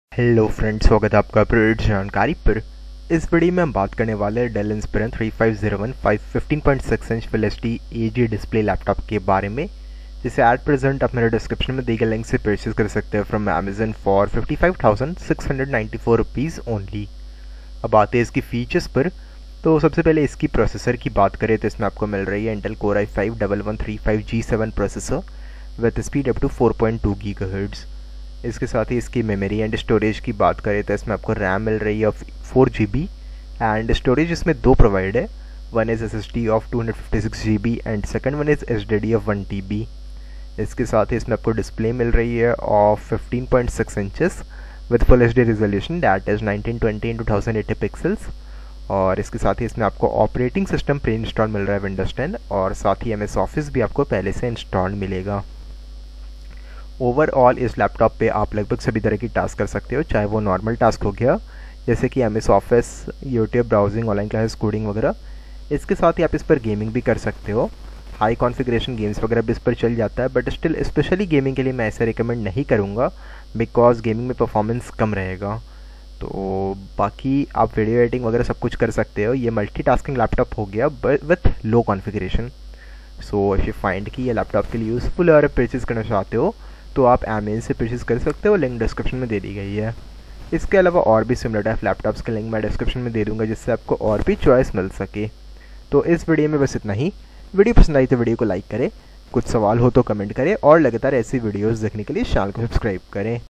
Dell Inspiron 3501 15.6" FHD AG Display Laptop (i5/ 4GB/ 1TB HDD + 256GB SSD) D560488WIN9B reviews
Purchase from Amazon

This video gives you a brief detail and review of Dell Inspiron 3501 5.6" FHD AG Display Laptop (i5-1135G7/ 4GB/ 1TB HDD + 256GB SSD/ Integrated Graphics/ Win 10 + MS Office/ Black) D560488WIN9B.